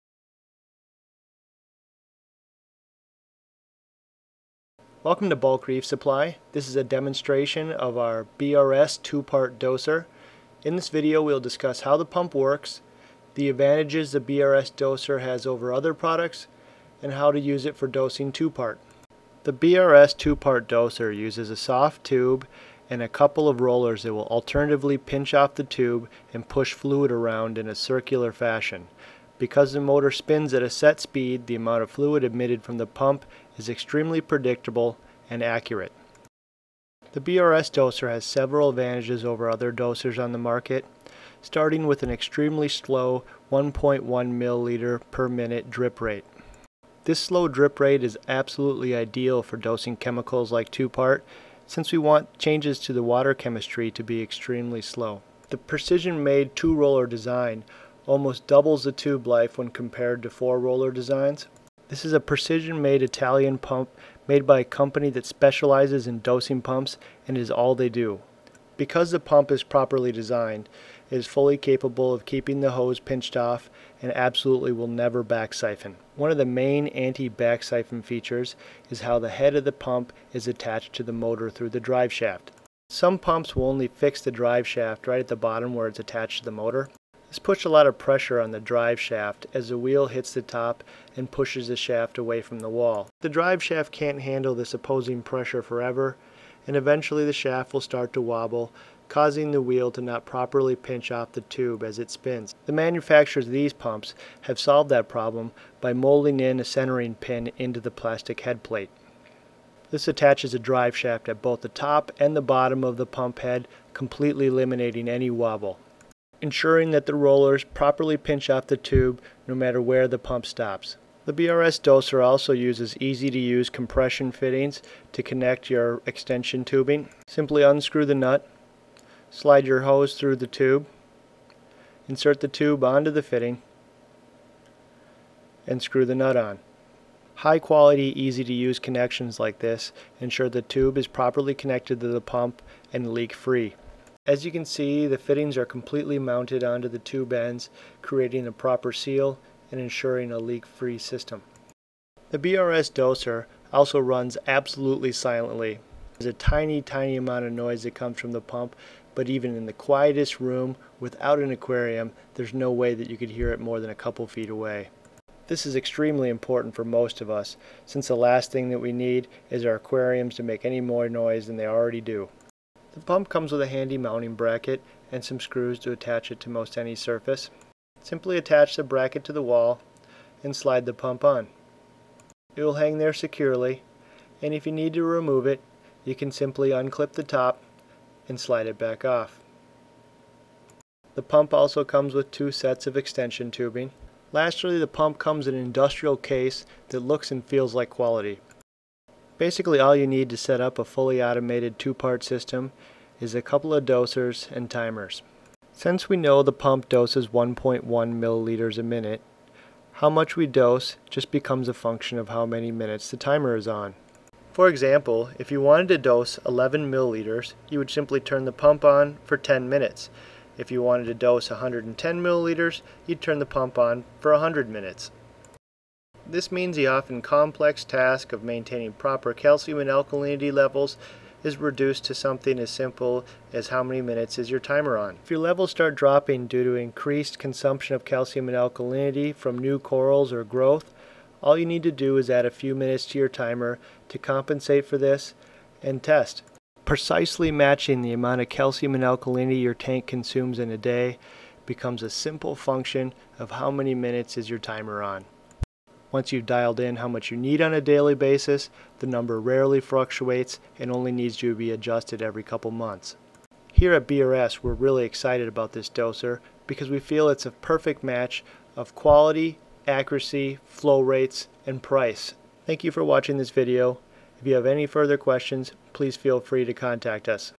BRS 2 Part Peristaltic Dosing Pump - Bulk Reef Supply
--- This video explains and illustrates the benefits of the BRS 2 Part Dosing Pump - Bulk Reef Supply